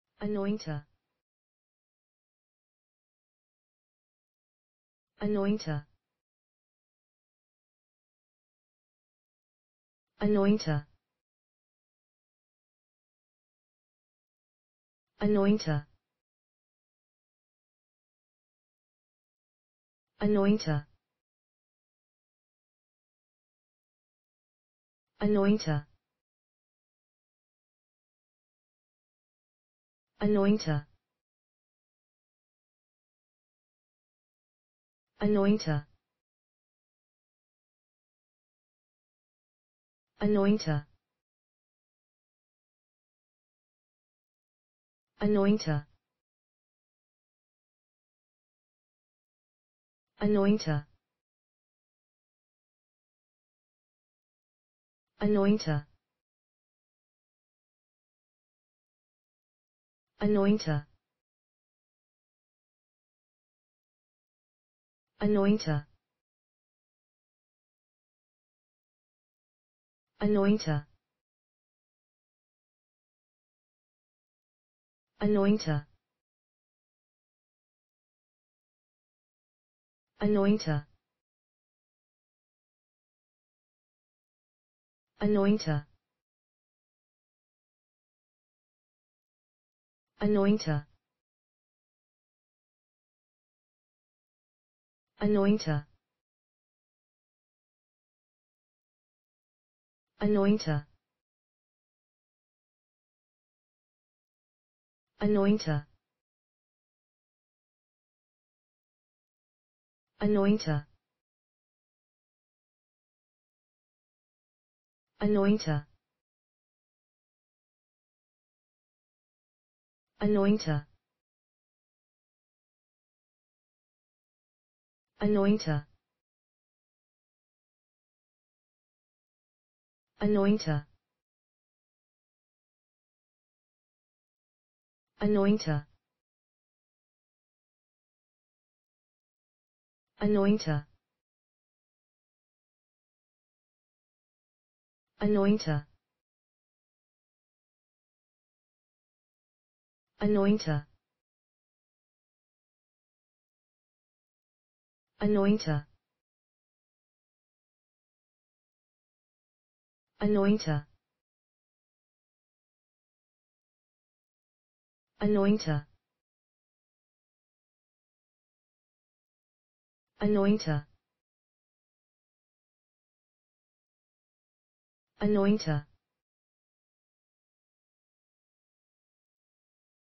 anointer: How to pronounce anointer in english (correct!).Start with A. Learn from me.
Follow me!Learn from me how to pronounce the word anointer in english.This video shows you how to pronounce the word anointer in english.

anointer

n.                one who anoints as a religious ceremony